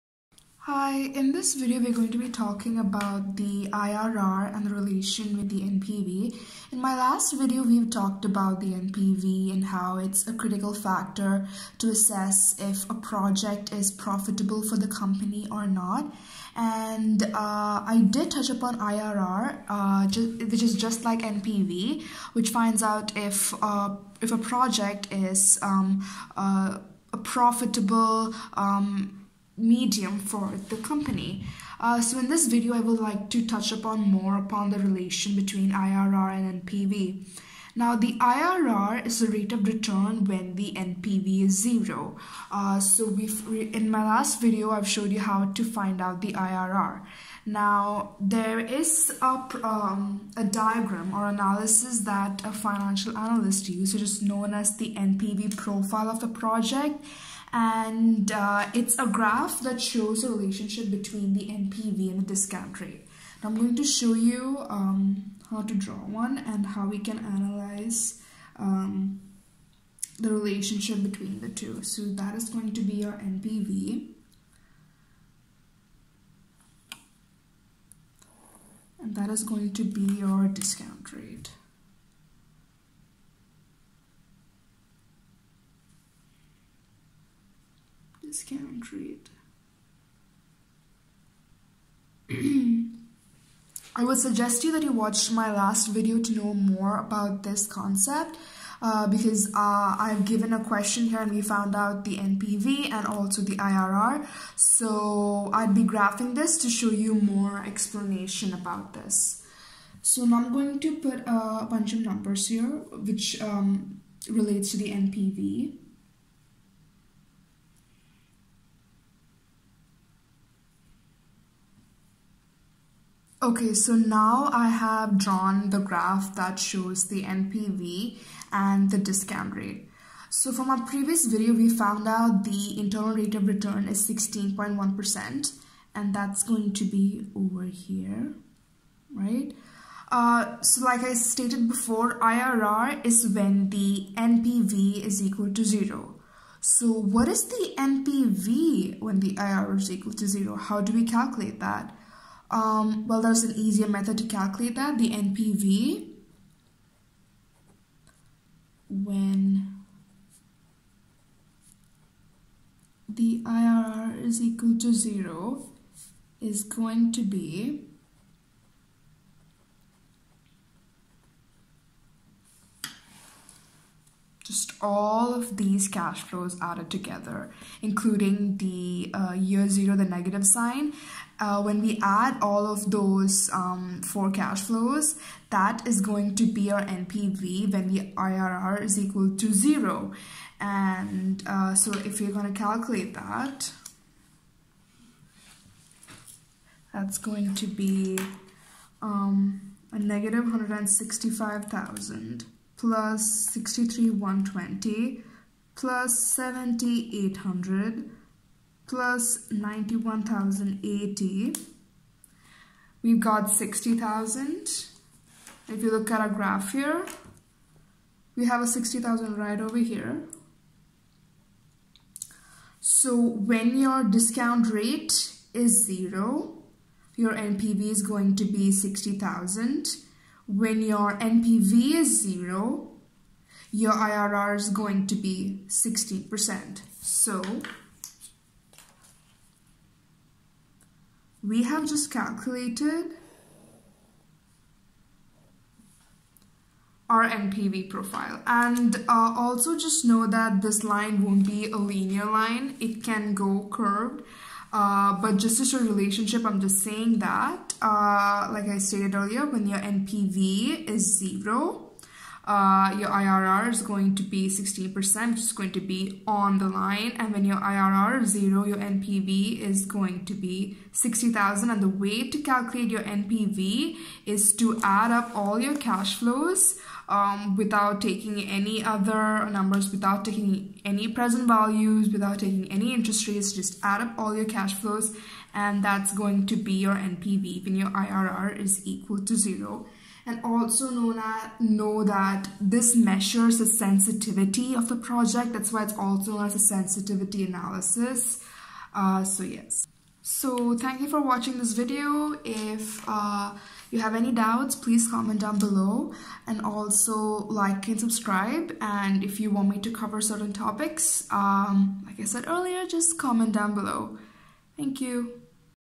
relationship between the NPV and IRR using the NPV Profile
in this video I touch upon the relationship b/w the IRR and NPV and the applications behind it. I'd suggest you watch my last video to see how i found out the NPV and IRR.

You could also find updates for my next video on my instagram page which is: